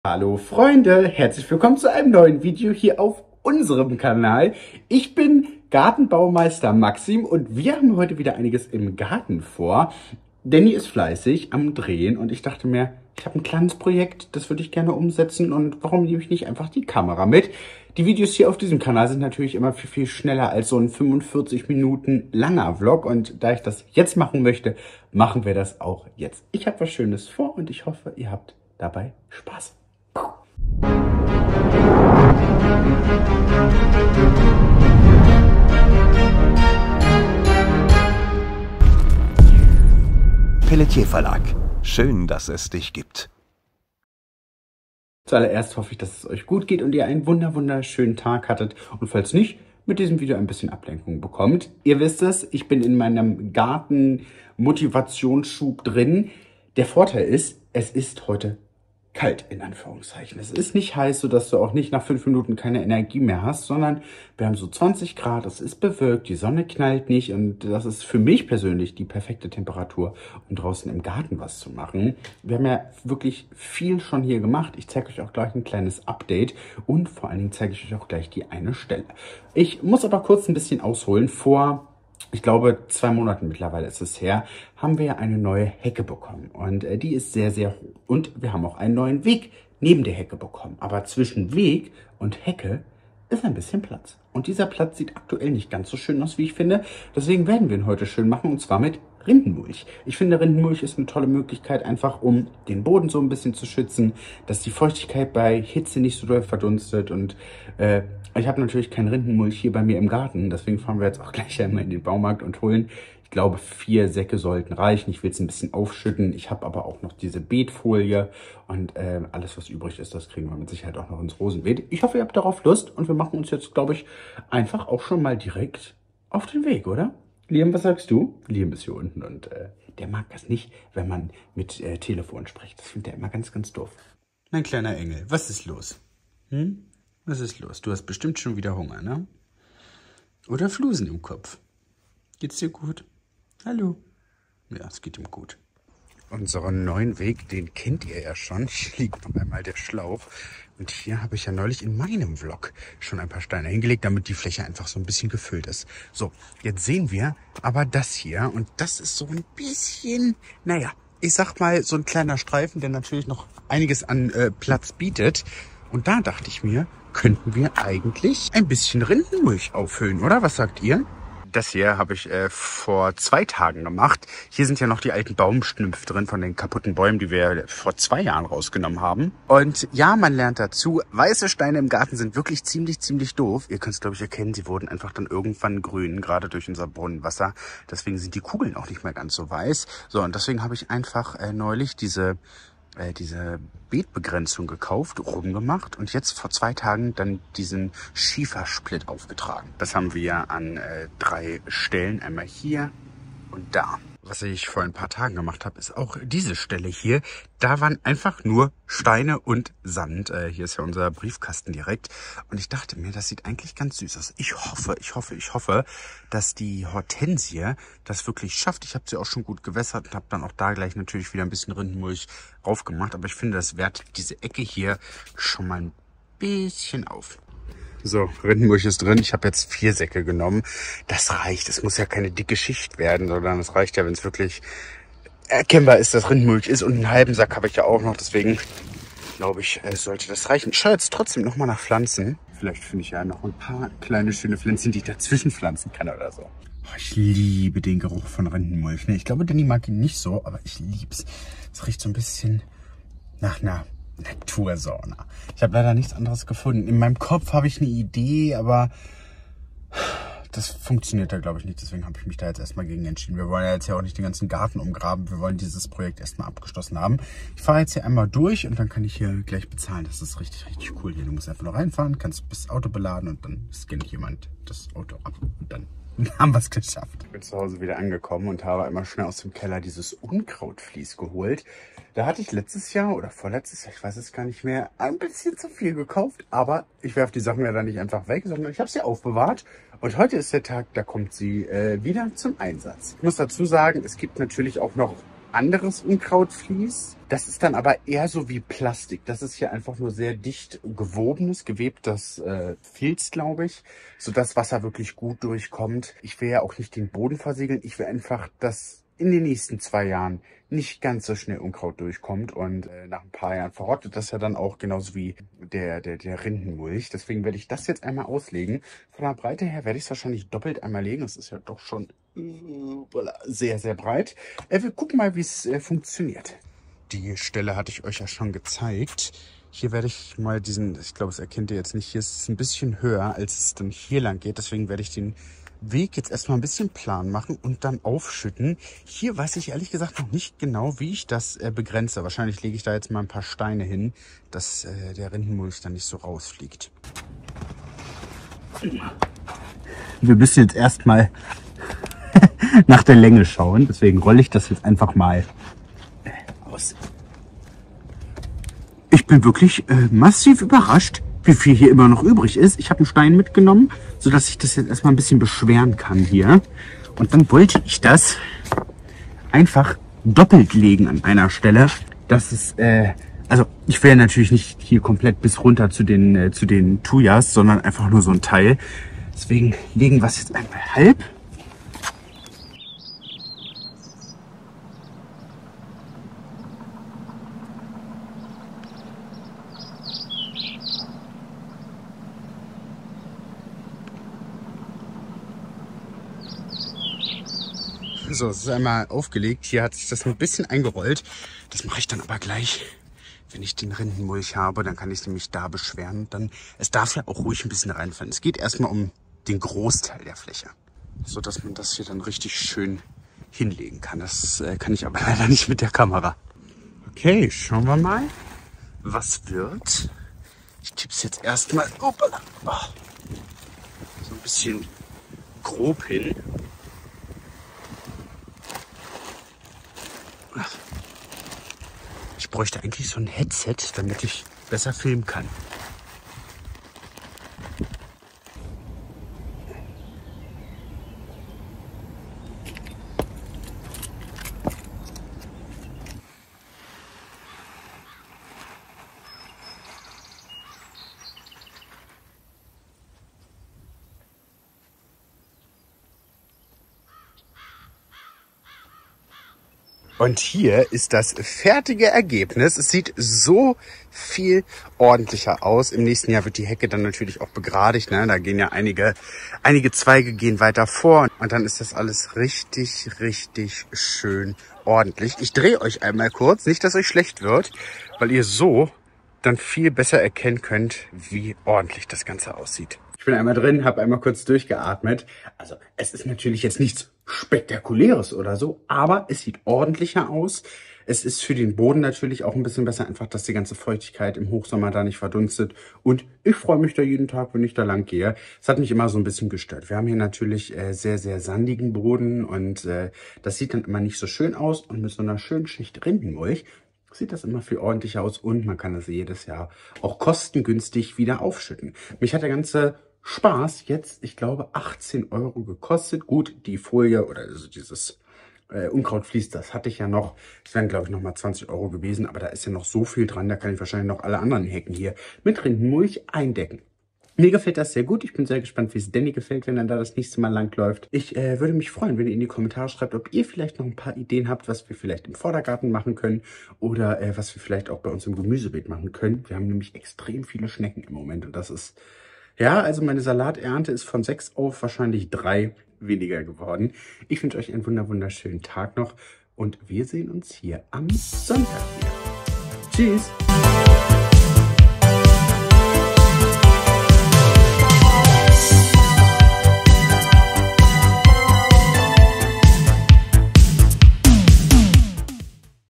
Garten UPGRADE - JETZT ist es endlich geschafft!
Maxikm und Denny haben es endlich geschafft. Es gibt neue Updates aus dem Garten! Viel spaß mit diesem video!